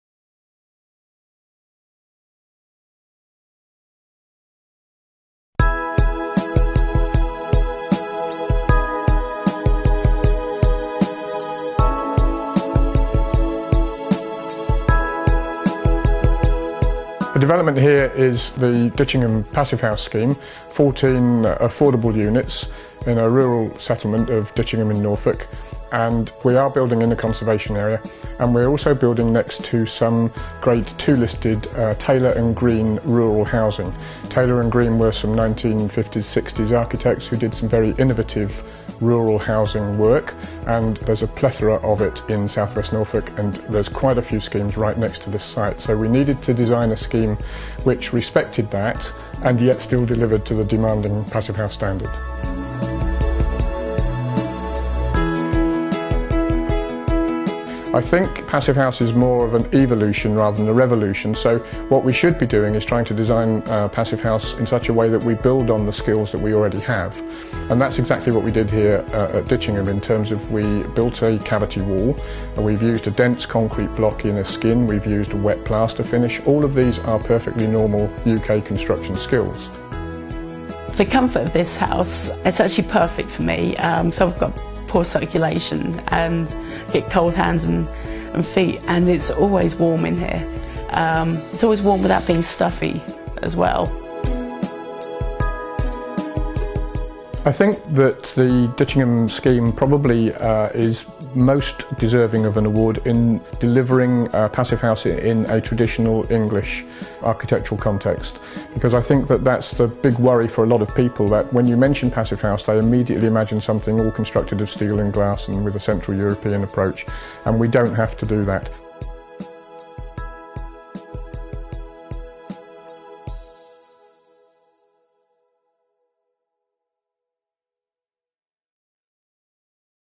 UK Passivhaus Awards 2013 - Ditchingham Passivhaus (Passive House)
The Ditchingham Passivhaus was a runner-up in the Social/Group Housing category of the UK Passivhaus Awards 2013.

The video was produced by UK content creation company, Regen Media